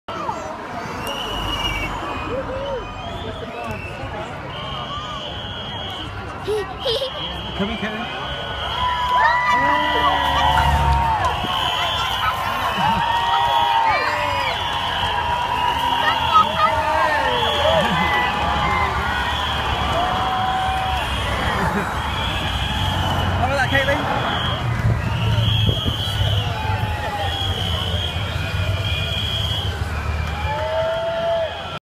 Tour de France 2014 Stage 3 Epping Sprint
Video taken just after Jean Marc Bideau beats Jan Barta in the intermediate sprint of stage 3of the 2014 Tour de France, Epping, Essex. 7 July 2014